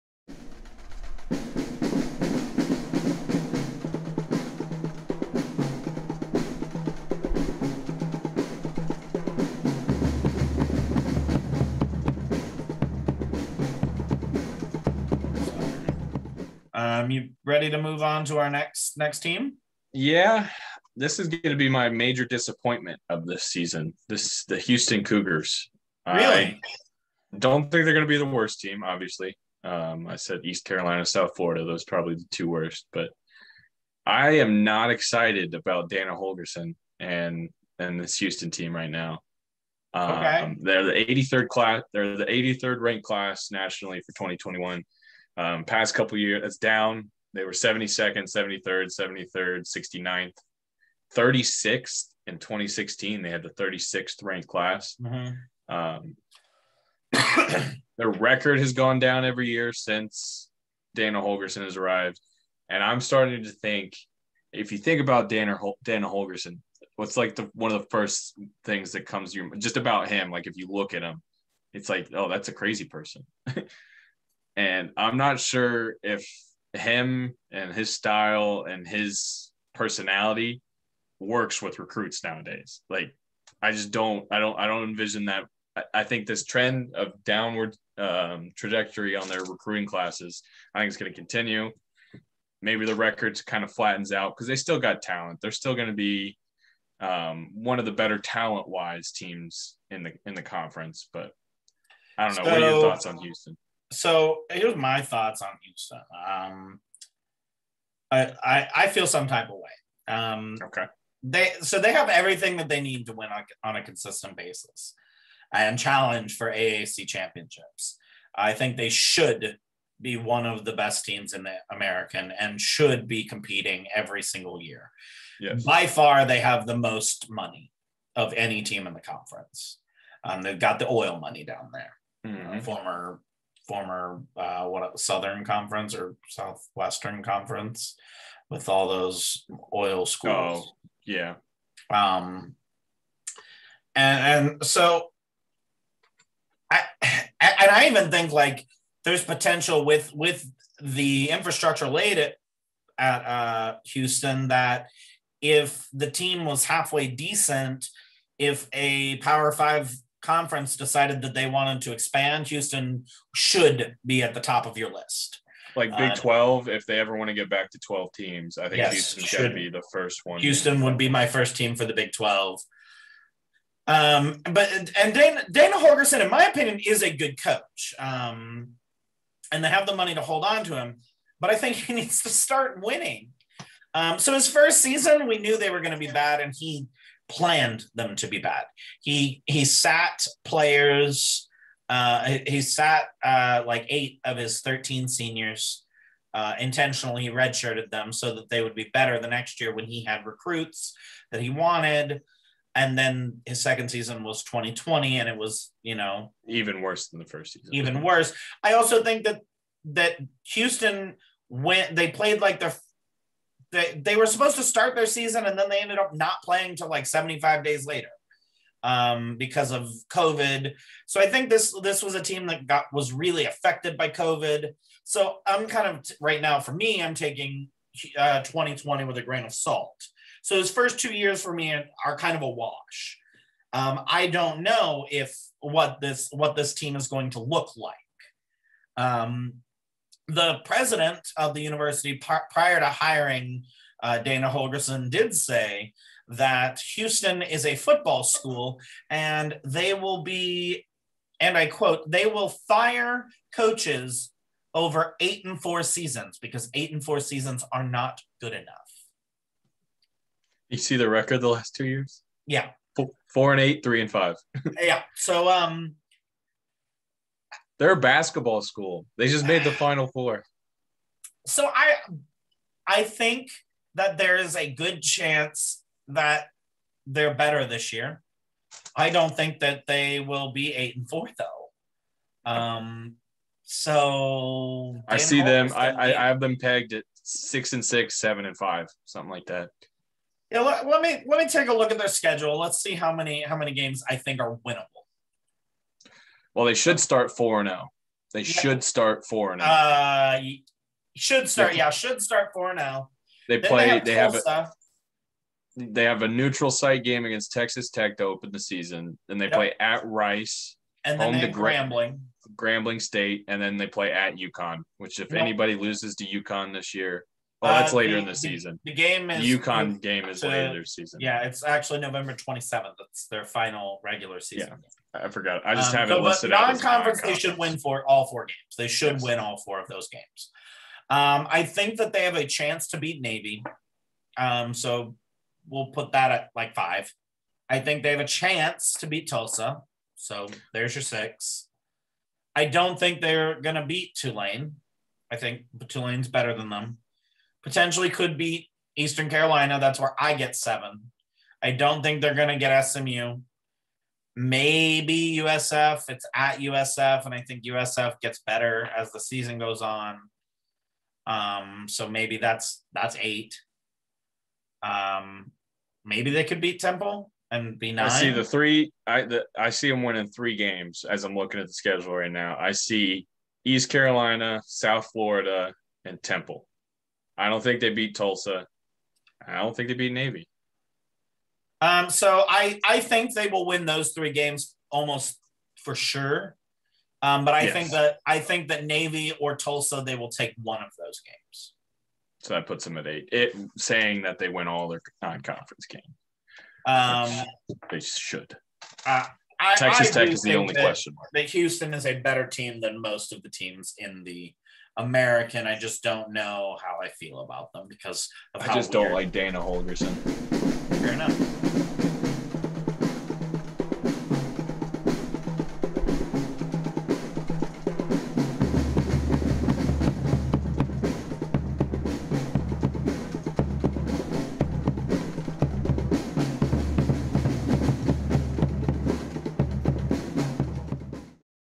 Houston Cougars - from ep. 10 State of the Conference: AAC
Our discussion on the Houston Cougars, where is Dana Holgorsen taking this team?

Dan's Twitter: @djsurfus12

Cody's Twitter: @A_POTATOE_SKIN